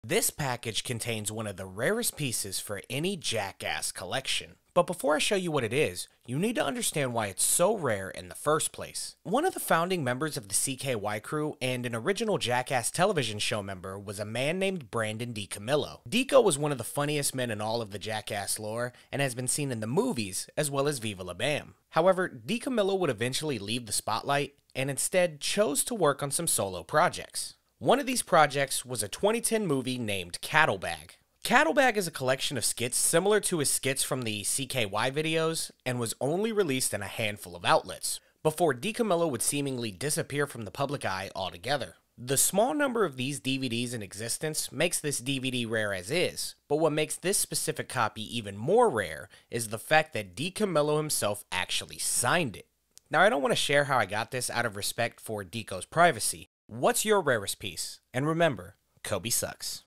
The Rarest Piece Of Jackass Media - Cattlebag - COBYSUCKS! Shorts
Jackass has plenty of related media for any collector, but one DVD created by Brandon DiCamillo might just be one of the hardest to find…